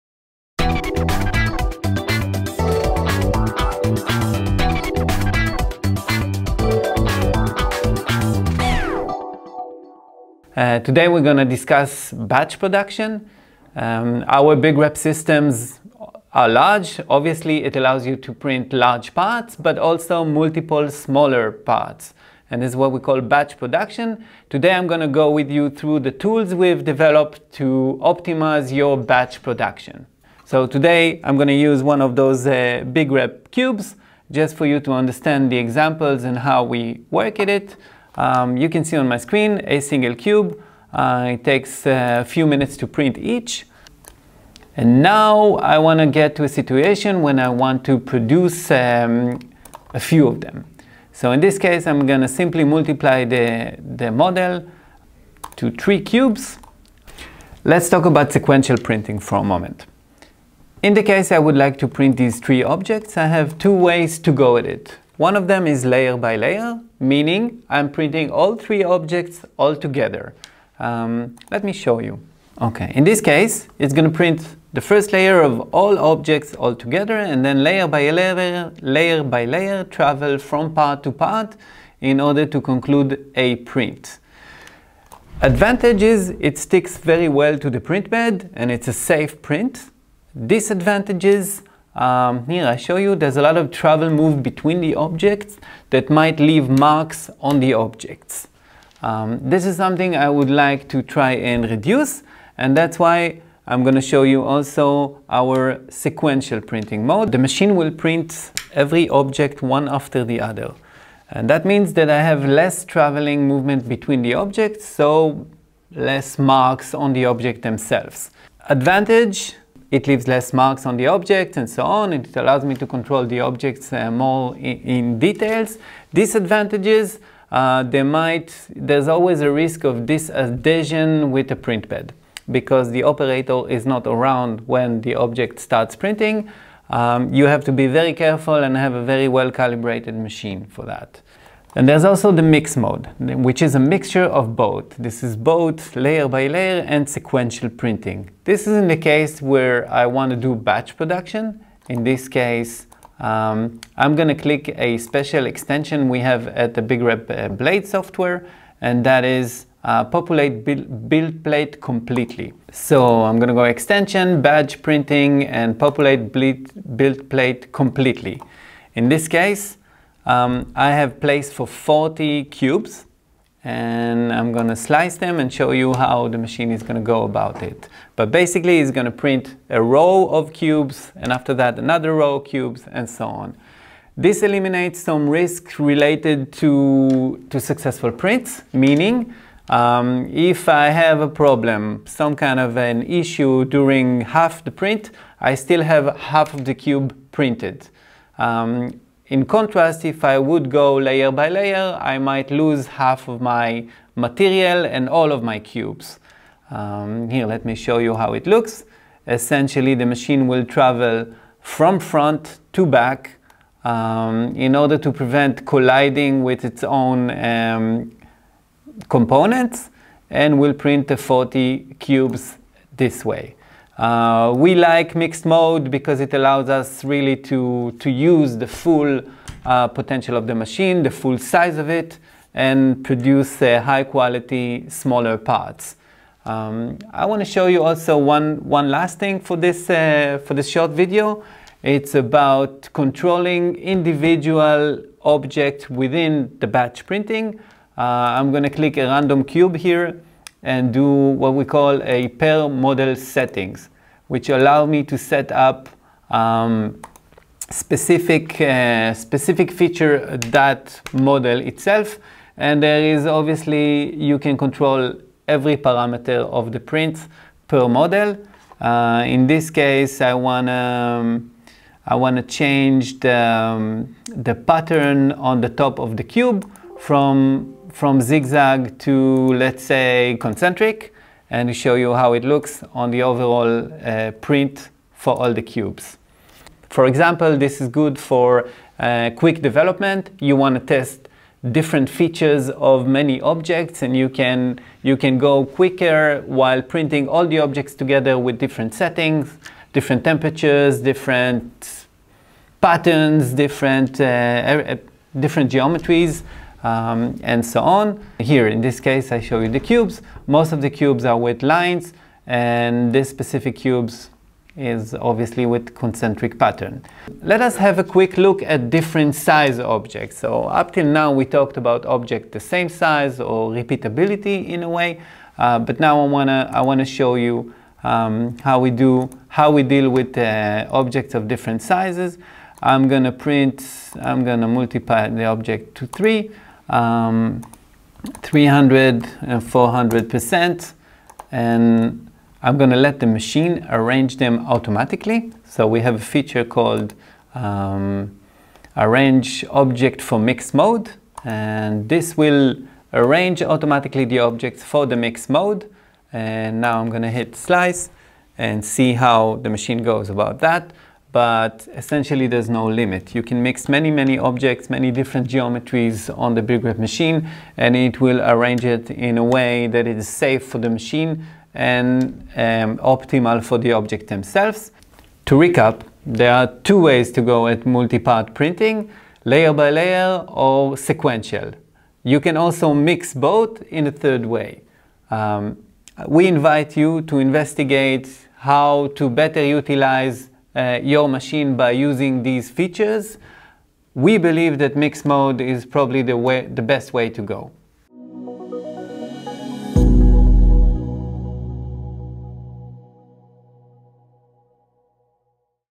Batch 3D Printing on a Large Format 3D Printer
With a large-format 3D printer, you can not only print large parts. You can also print a lot of smaller parts in one go.
Michel from BigRep explains how to get the most parts out of your 3D Printer and how to make sure you will get the highest quality.